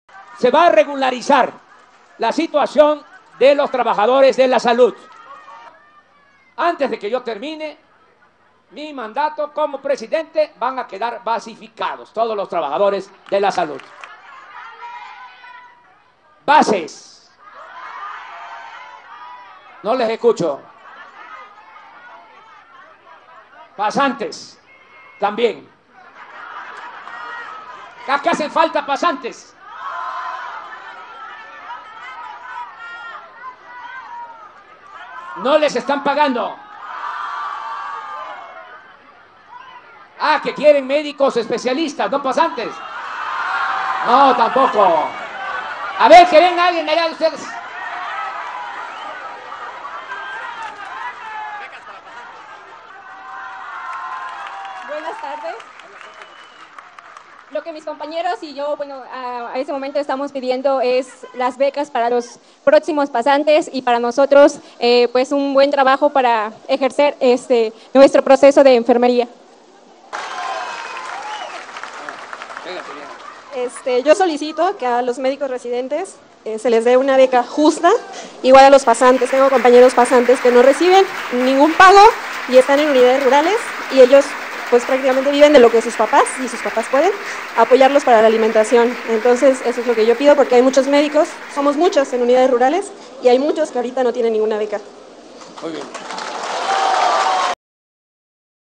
No sólo hay que sentarse a aplaudir el discurso del político, también hay que exigirle.
Bien por los médicos y enfermeras que exigen mejores condiciones de trabajo y becas para los pasantes. No sólo es sentarse y aplaudir el discurso repetitivo de Andrés Manuel López Obrador, también hay que exigirle 👊🏼👍🏼👏🏼